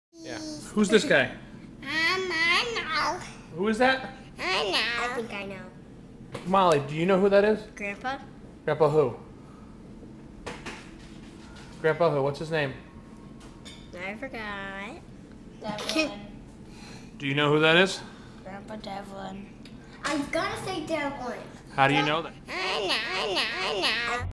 Who is Grandpa?
Brodie and the rest of the kids try to figure out who the guy with the Porn-Star mustache is.